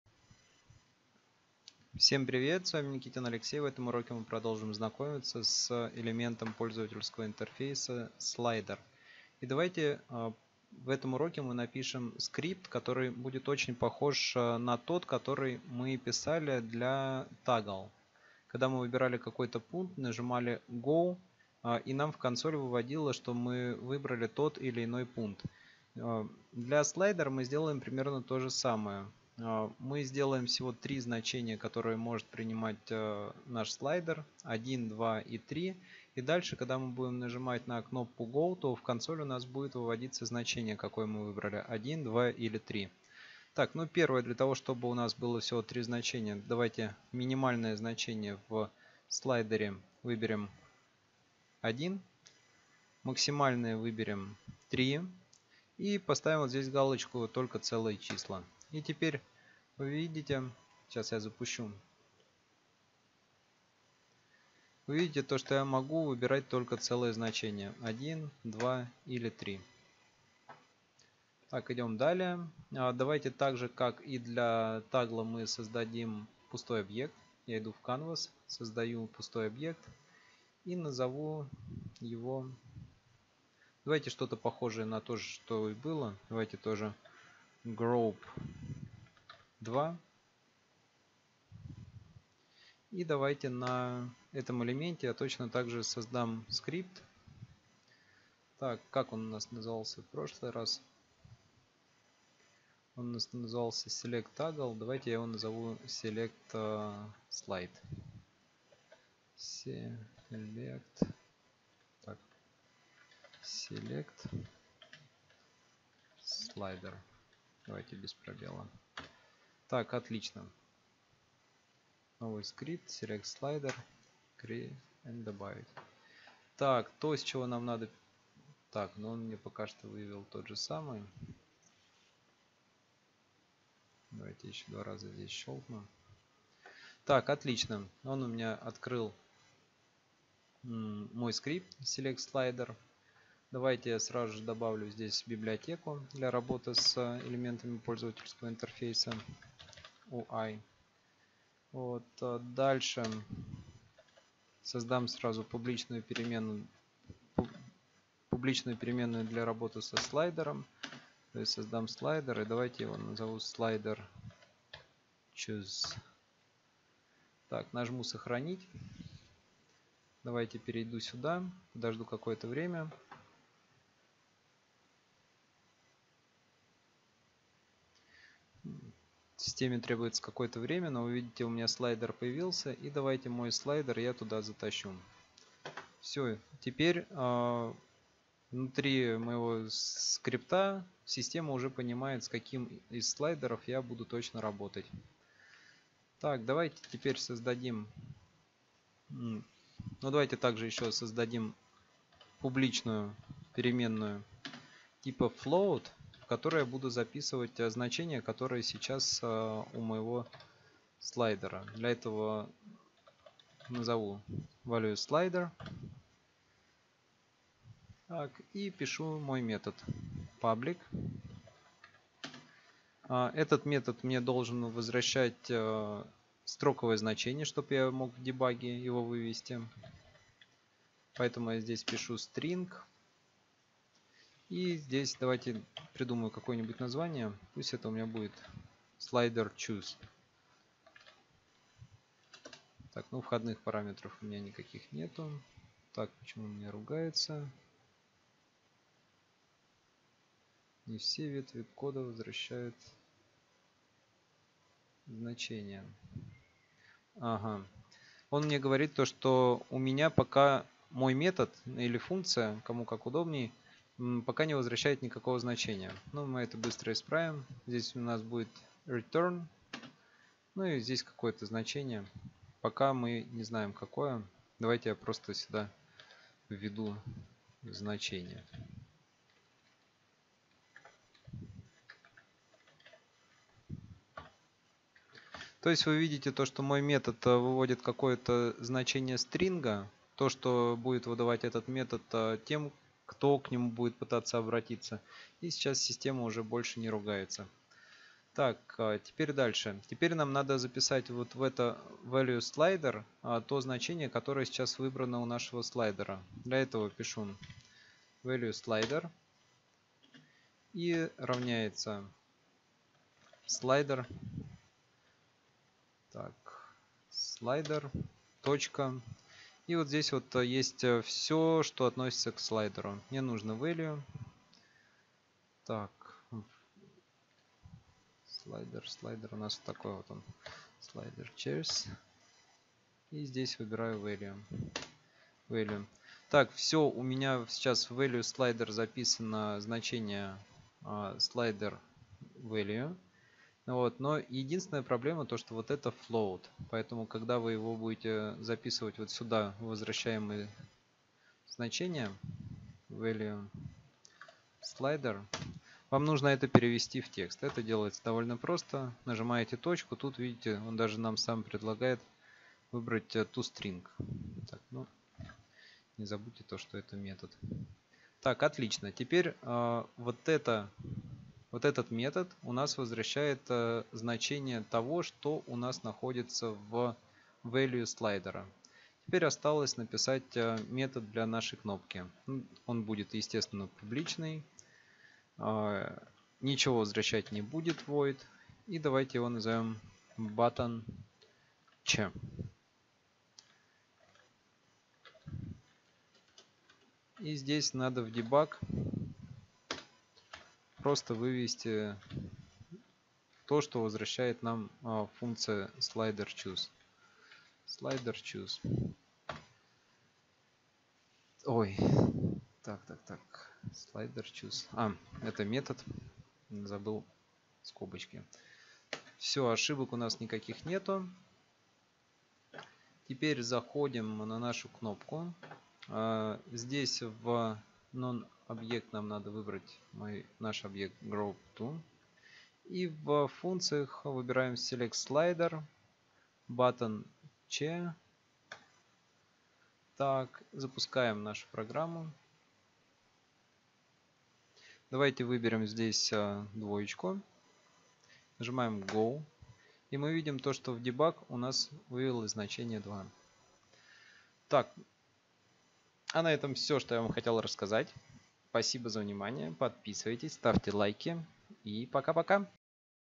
Unity3D Урок 10 (часть 2) Пользовательский интерфейс UI Slider
В этом уроке мы познакомимся с элементом пользовательского интерфейса UI Slider